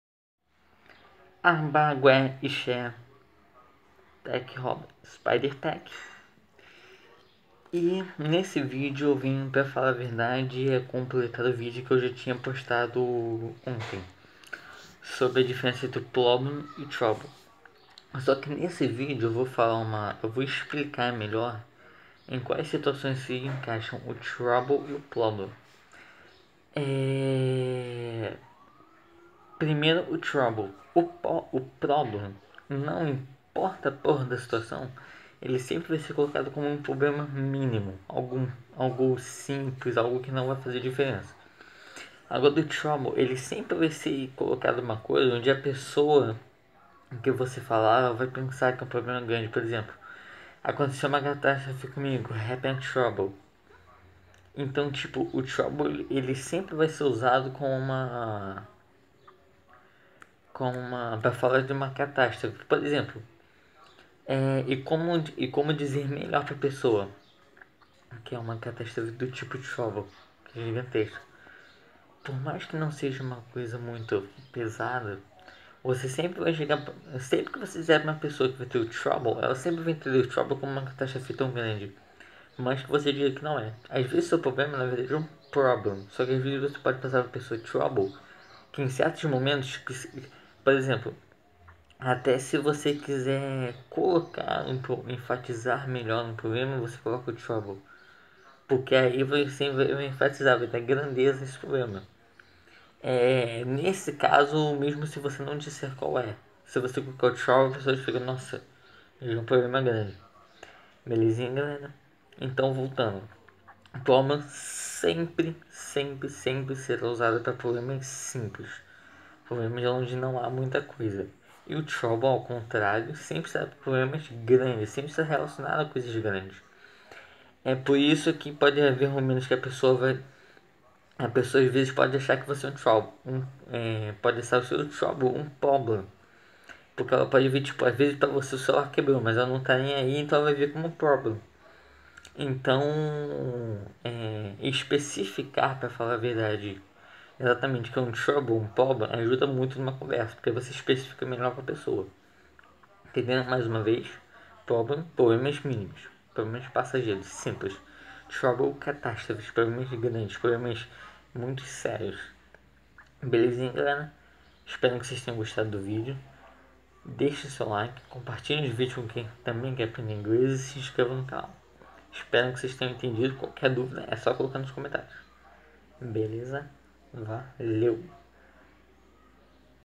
theenglishclass2 - como usar trouble e problem numa conversa!!
Nesse vídeo, eu explico como usar o trouble e problem numa conversa e como é importante usar o termo certo na conversa. Lembrando, qualquer dúvida, deixem nos comentários. Espero que curtam!!!
( conteúdo passado a mim pelo prof danilo nicolas).